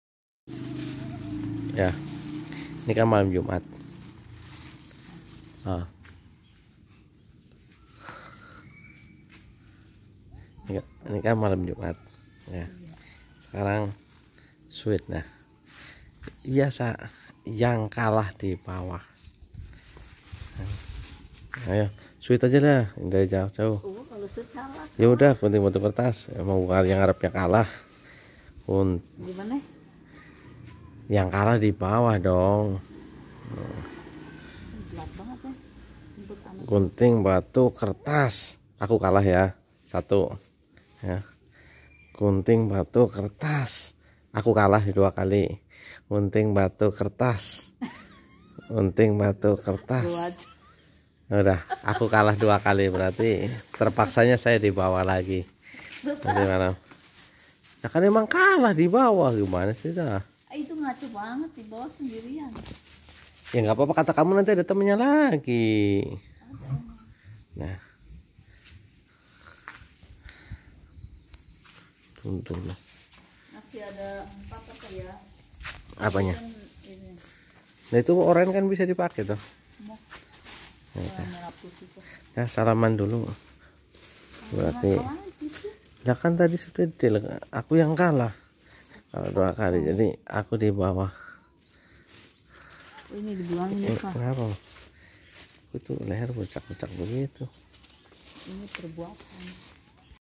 Suit dan Gunting Batu Kertas 06 Papa mama menentukan posisi jadwal malam jumat
Permainan ini bisa bikin seru dan bahagia keluarga saling berinteraksi